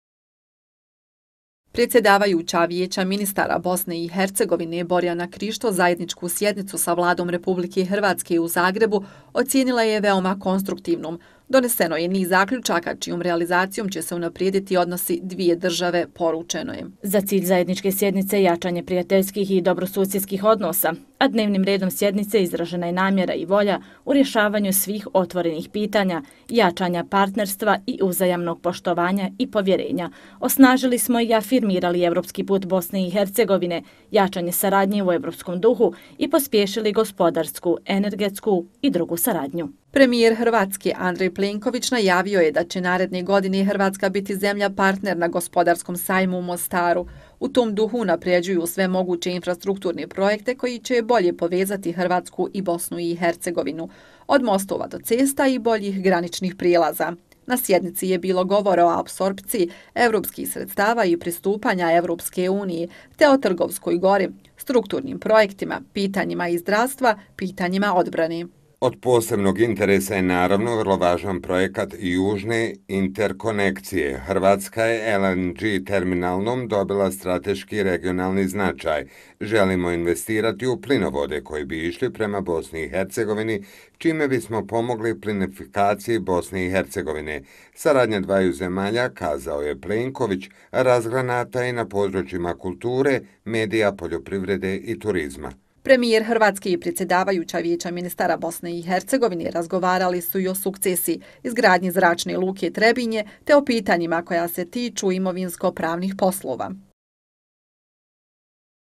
Zajednička sjednica Vlade Republike Hrvatske i Vijeća ministara BiH, održana je danas u Zagrebu
Zajednička sjednica Vlade Republike Hrvatske i Vijeća ministara Bosne i Hercegovine, održana je danas u Zagrebu. Na dnevnom redu zajedničke sjednice je niz zaključaka od zajedničkog interesa Bosne i Hercegovine i Hrvatske kojima će se dodatno unaprijediti bilateralni odnosi. Prije sjednice održan je sastanak Borjane Krišto, predsjedavajuće Vijeća ministara BiH i Andreja Plenkovića, predsjednika Vlade Republike Hrvatske. Nakon sjednice upriličeno je potpisivanje sporazuma.